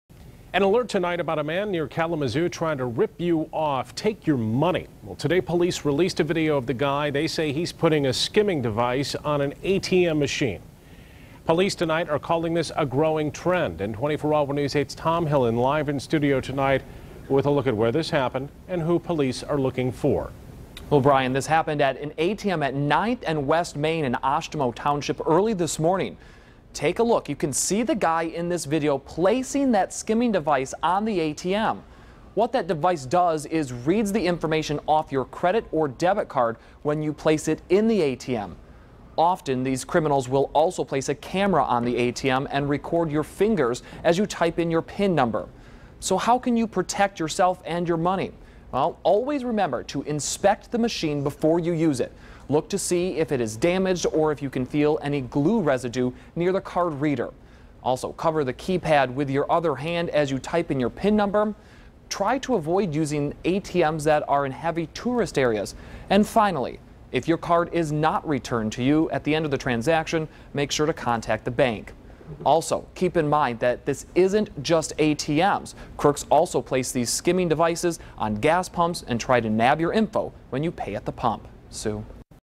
ATM skimming suspect caught on tape
The Kalamazoo County Sheriff's Office is trying to track down a man who is believed to have installed a skimming device onto an ATM machine in Oshtemo Township.